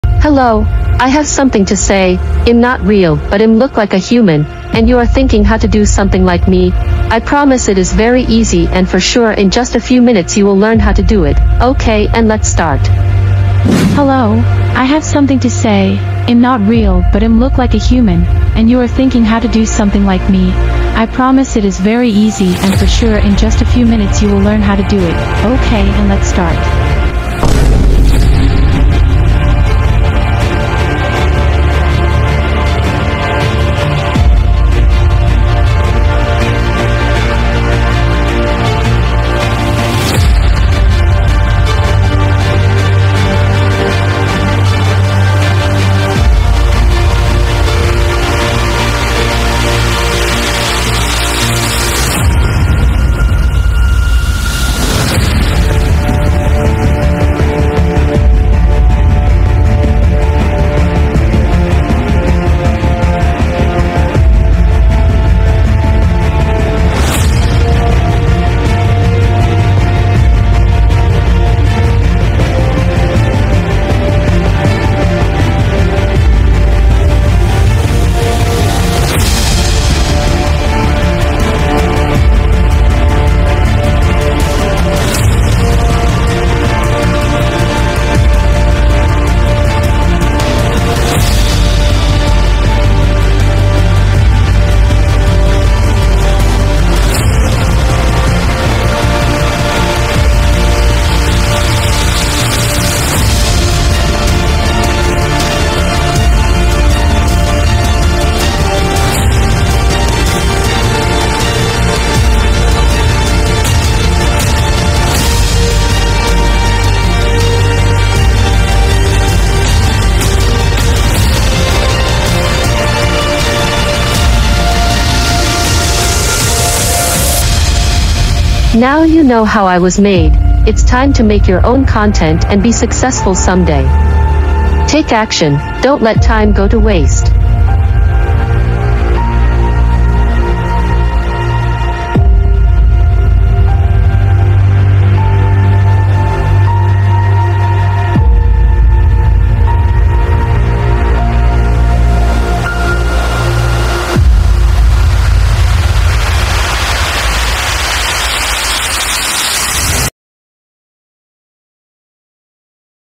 I FOUND A NEW WAY TO MAKE A VIRAL REELS and YOUTUBE SHORTS! easy way!
Discover the ultimate secret to crafting captivating viral Reels and YouTube Shorts effortlessly! Unveil a groundbreaking approach that guarantees engagement and success, revolutionizing your content creation journey. With this innovative method, creating viral-worthy content becomes not just achievable, but a thrilling adventure waiting to unfold. Say goodbye to complexities and hello to simplicity as you embark on this easy yet powerful path to viral fame!